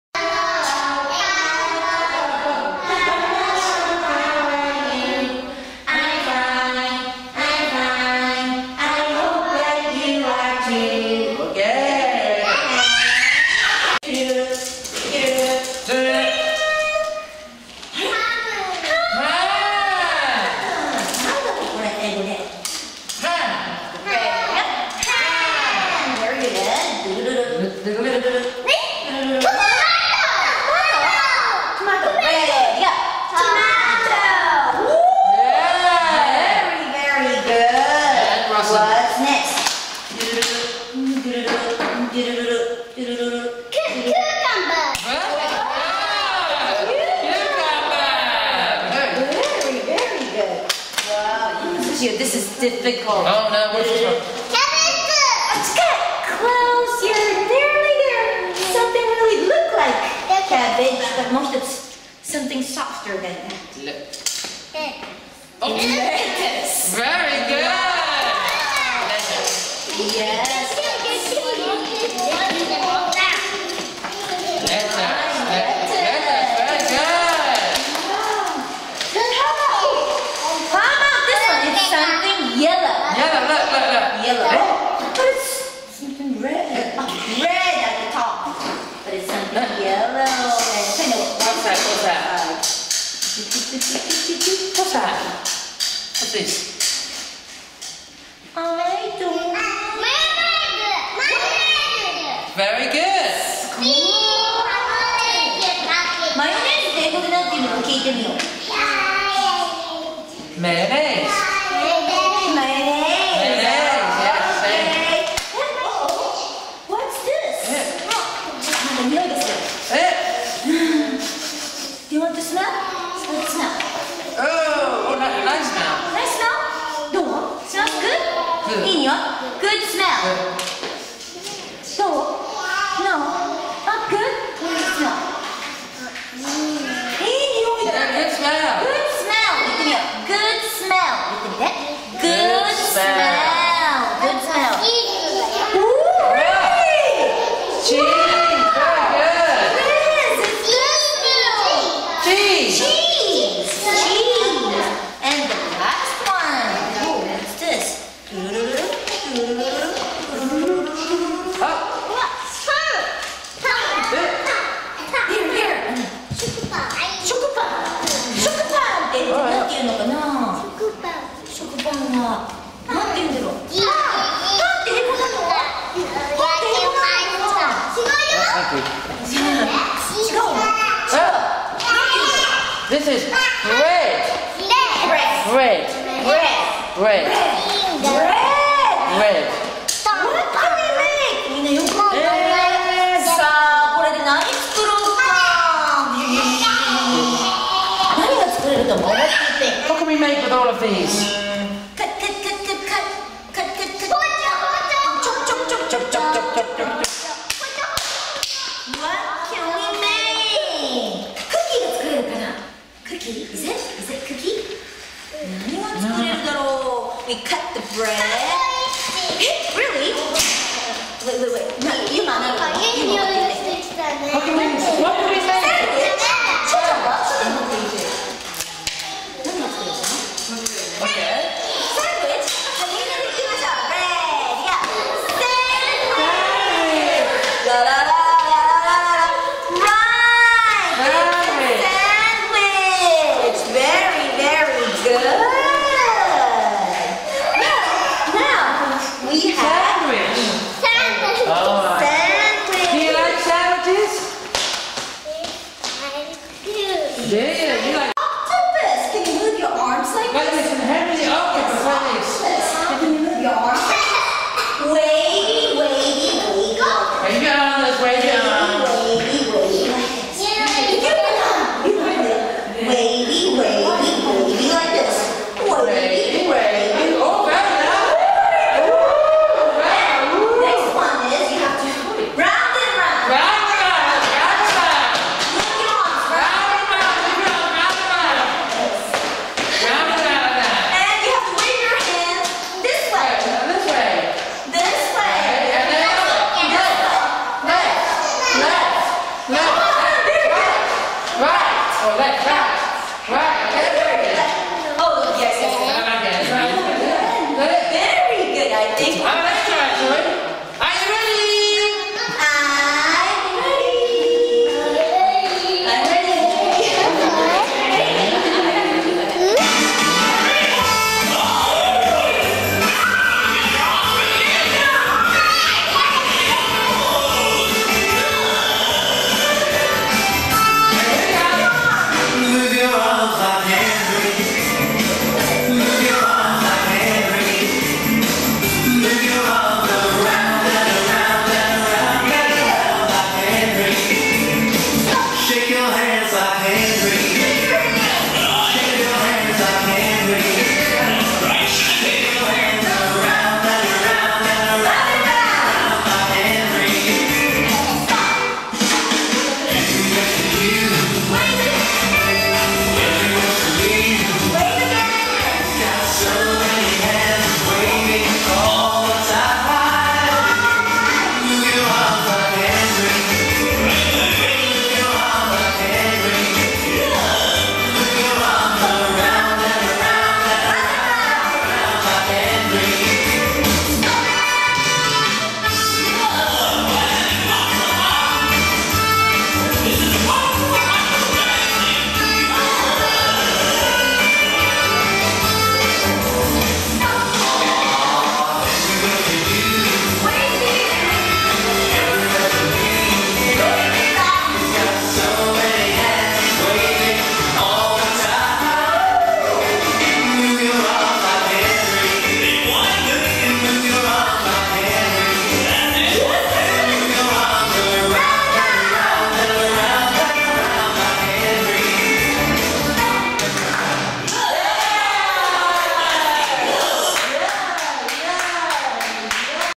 EnglishHouse英語でスポーツ１１月号ダイジェスト16：45～クラス
イングリッシュハウスが三里塚にて開講している
英語でスポーツの１１月号　ダイジェスト版　16：45～クラスのようす